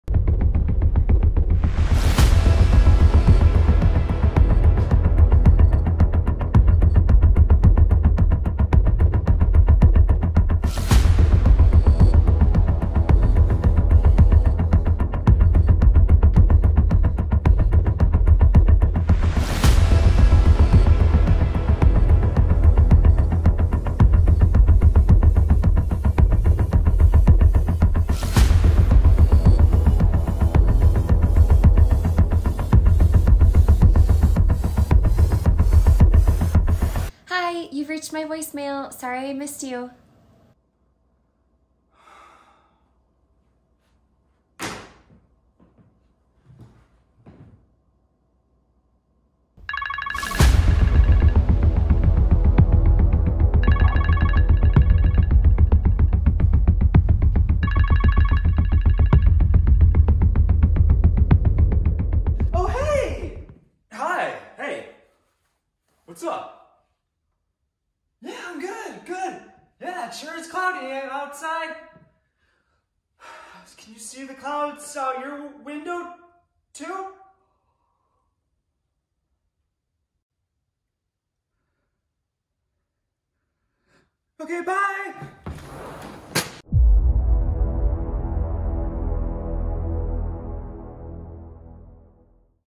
When You’re Nervous to Call Your Crush.
When your want to call your crush but you're so nervous that you end up just pacing around the house for a half hour.